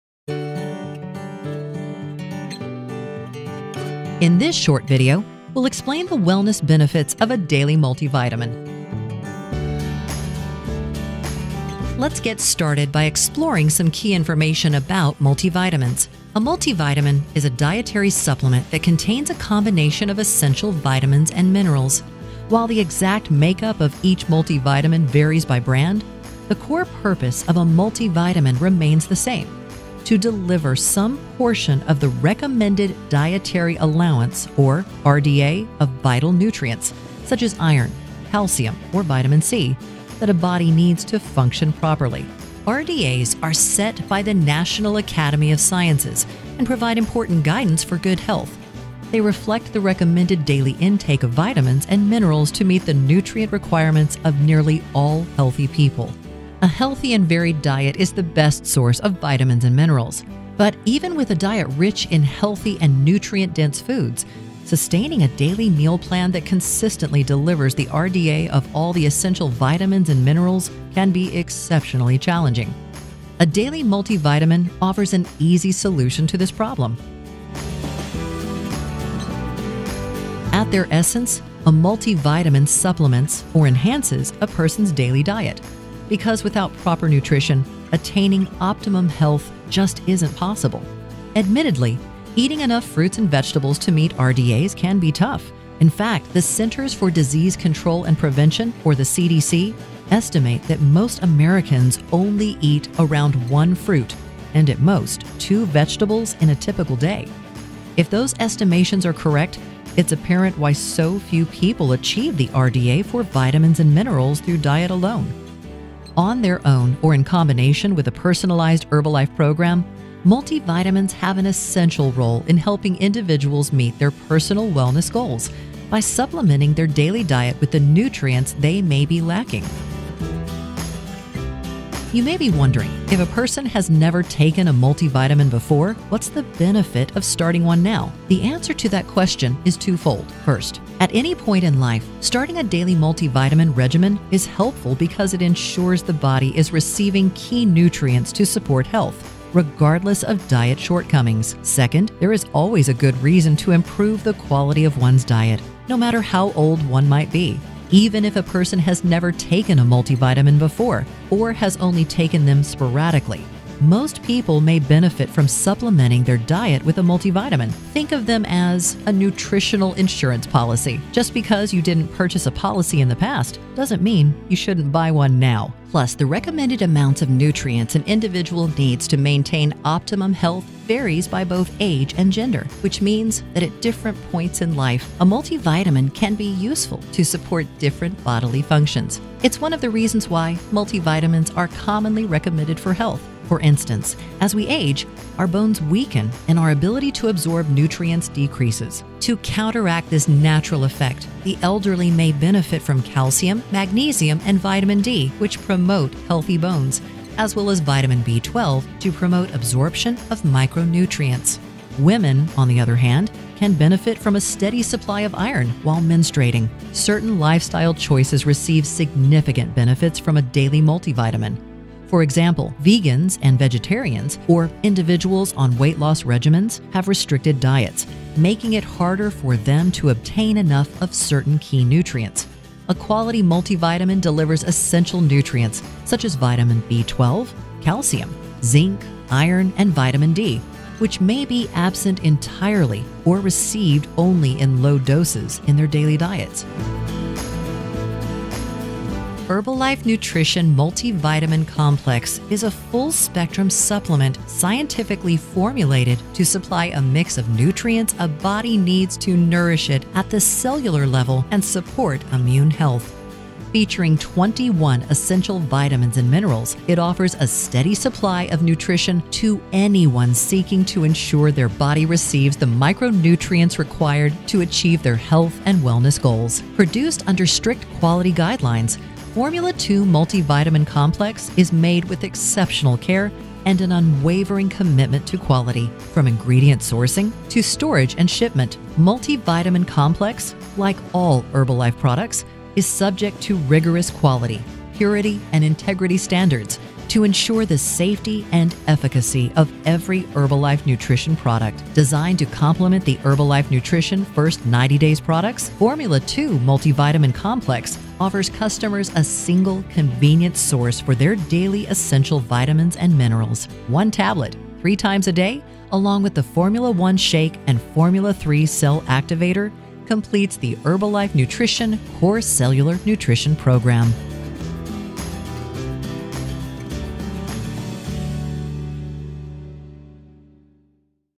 Multivitamin Mineral & Herbal Tablets
PRODUCT OVERVIEW:
Essential vitamins, minerals and antioxidants, supports your immune system. A daily vitamin – mineral supplement provides the best for your wellness goals. High quality multivitamin supplements to ensure you get all the essential vitamins, minerals and nutrients your body needs.

KEY FEATURE:
1. Provides a unique blend of 23 essential vitamins, minerals & herbs.

PRODUCT USAGE:
One tablet of Formula 2 should be consumed three times a day along with your meals.